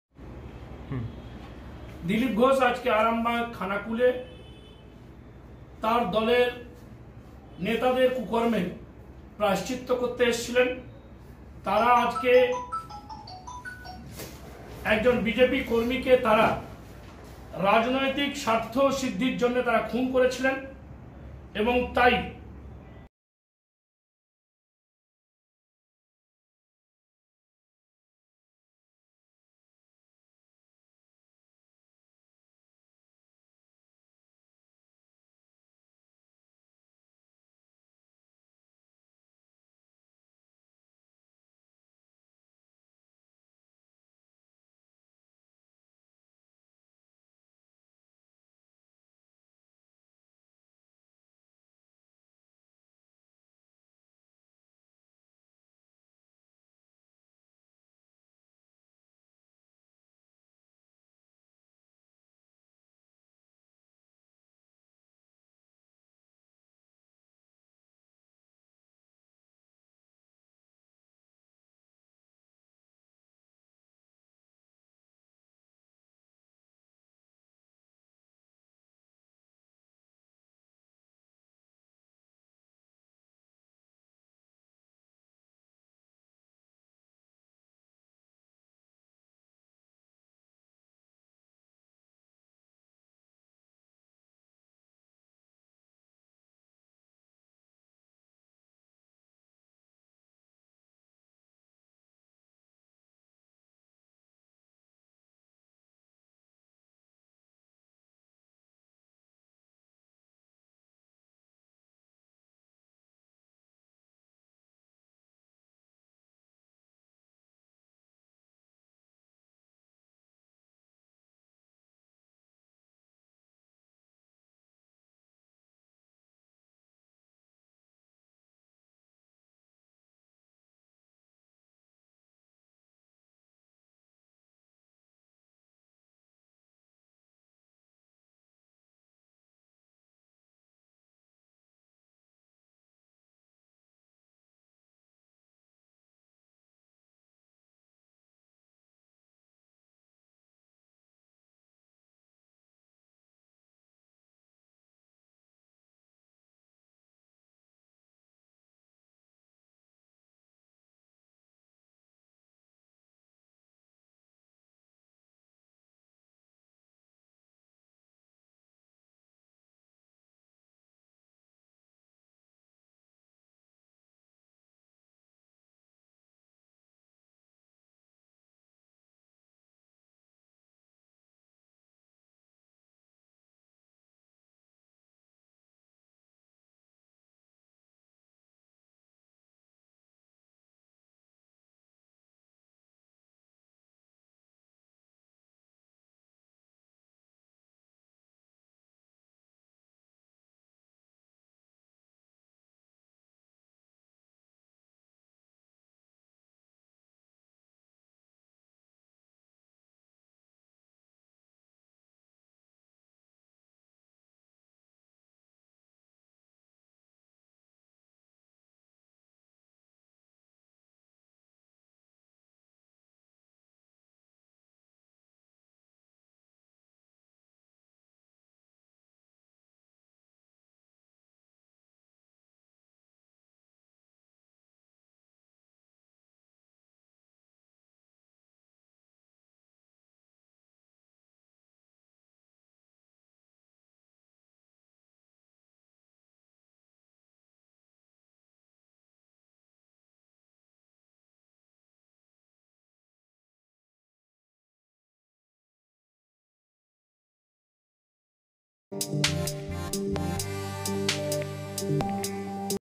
দলের নেতাদের কুকর্মের প্রায়শ্চিত্ত করতে খানাকুলে এসেছিলেন দিলীপ ঘোষ: তৃণমূল জেলা সভাপতি দিলীপ যাদব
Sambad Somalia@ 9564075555
 নিজের দলের নেতাদের কুকর্মের প্রায়শ্চিত্ত করতে খানাকুলে এসেছিলেন বিজেপি রাজ্য সভাপতি দিলীপ ঘোষ: তৃণমূল জেলা সভাপতি দিলীপ যাদব